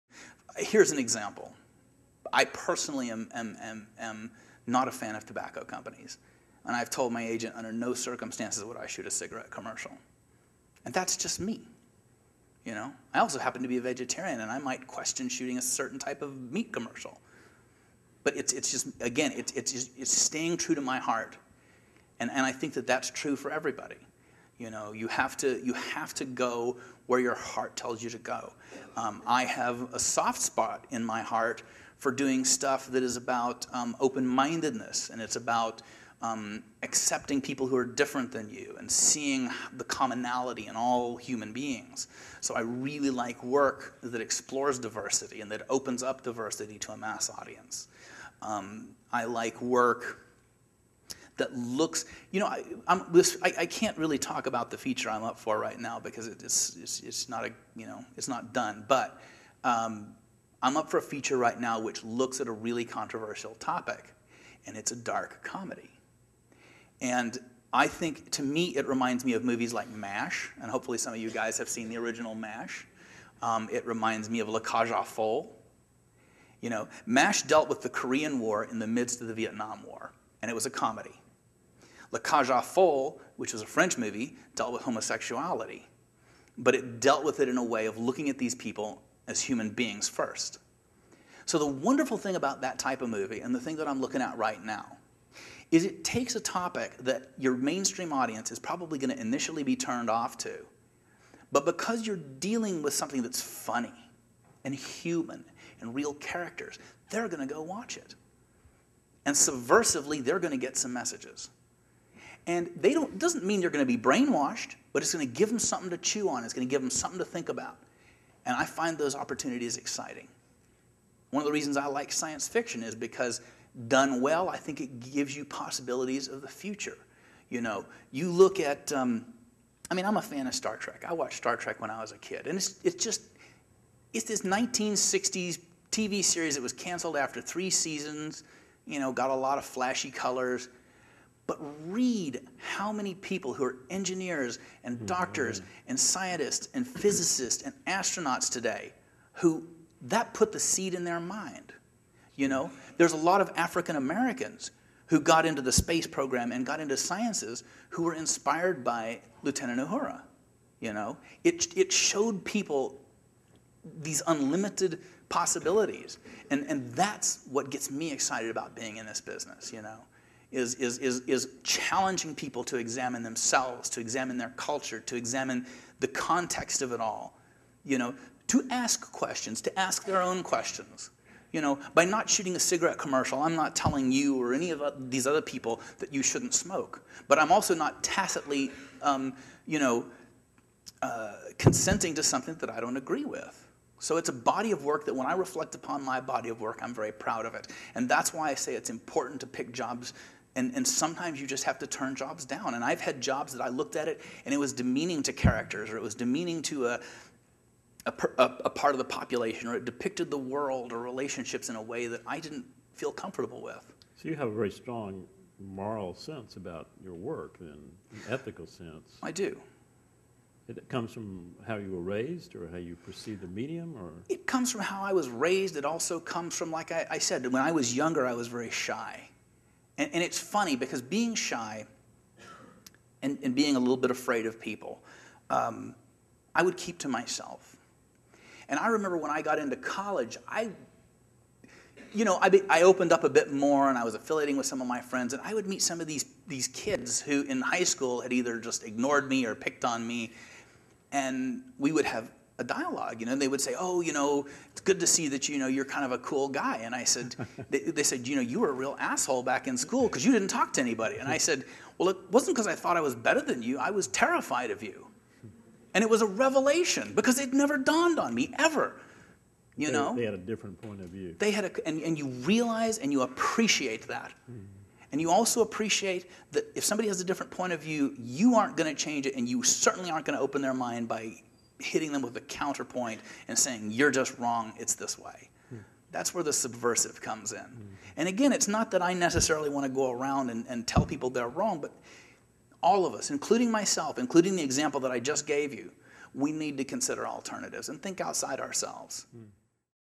Brad Rushing On Doing Work that is Meaningful to You
- In a forum at Loyola University, New Orleans, cinematographer Brad Rushing discusses why he chooses to work on the projects he accepts, and why it is important to do work that you not only feel okay about getting paid for, but that you believe in the message of too.